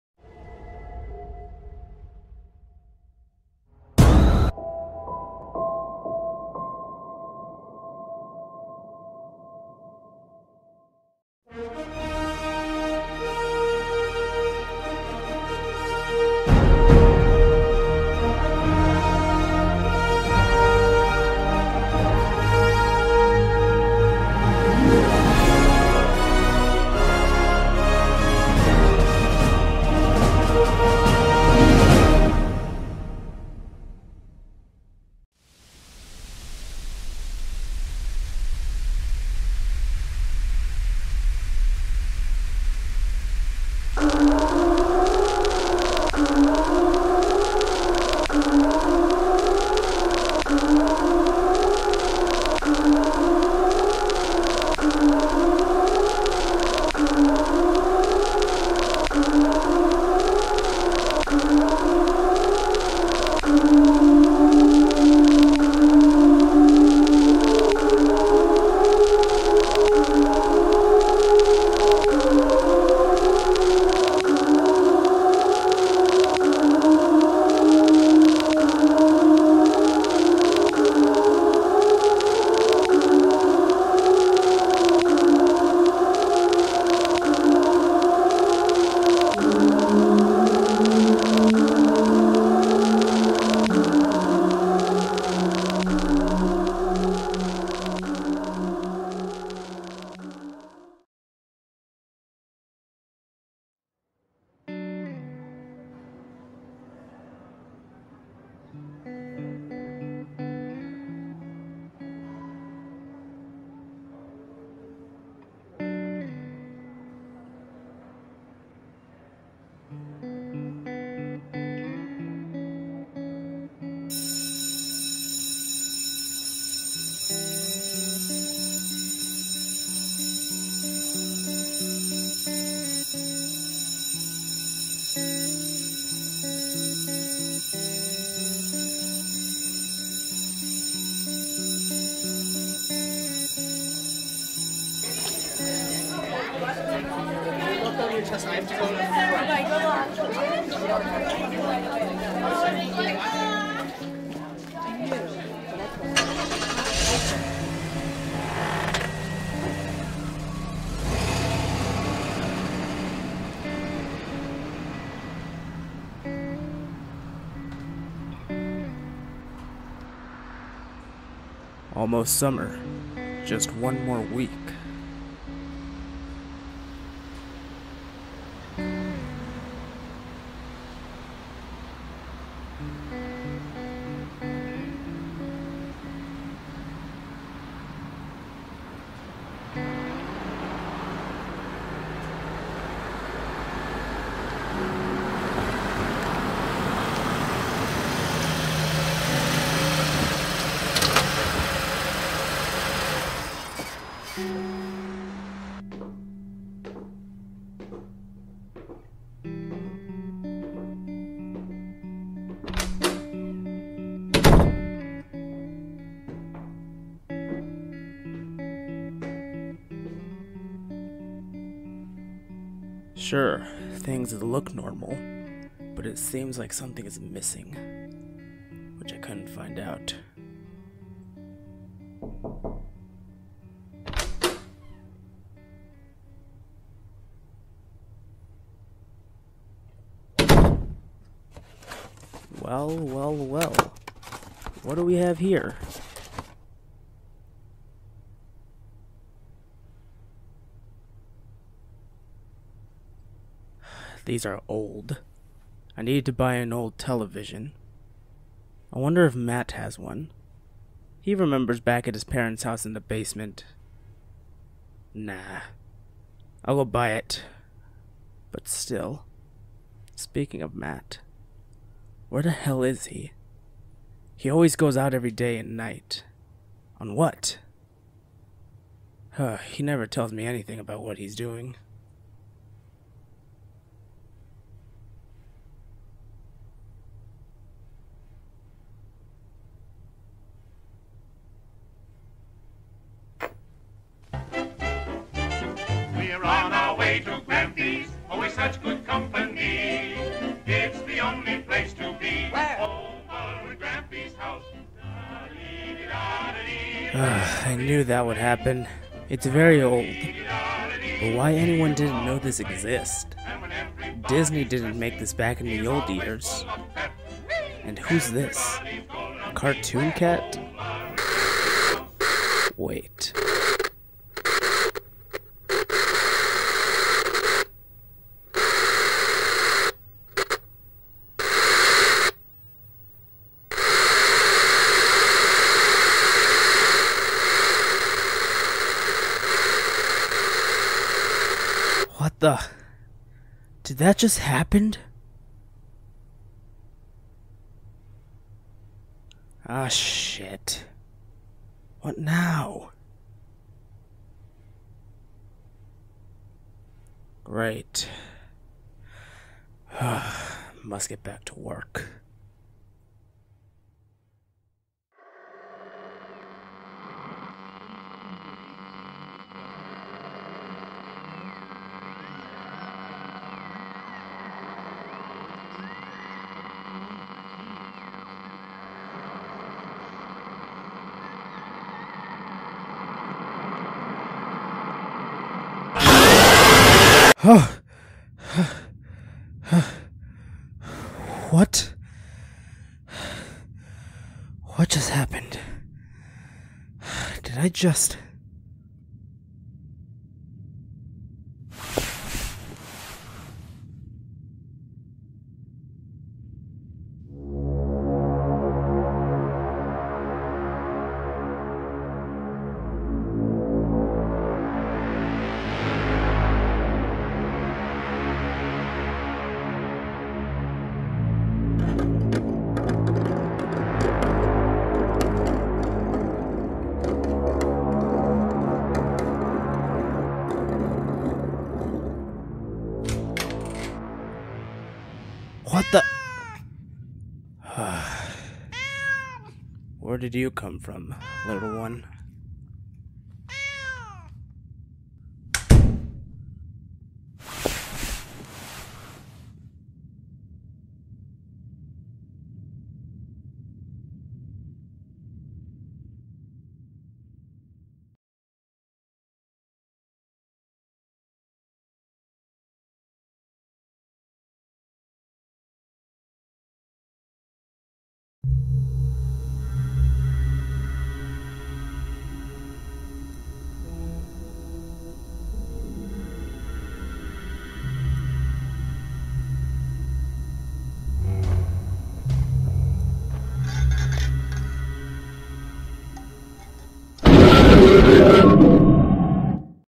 CARTOON CAT: The Minecraft Movie | PART 1
In the year 2013, a college student founds old tapes that will change his life... if he'll survive.

Cast: GreenWaveVoiceover

Cartoon Cat made by Trevor Henderson

Directed by Lord of Flames

Produced by Lord of Flames

Written by Lord of Flames

Animated by Lord of Flames

[WARNING! This video is NOT for kids! It contains flashes, loud noises, and languages]

I need a Song Composer for HALLOWED NIGHTMARE (A Creepypasta Movie)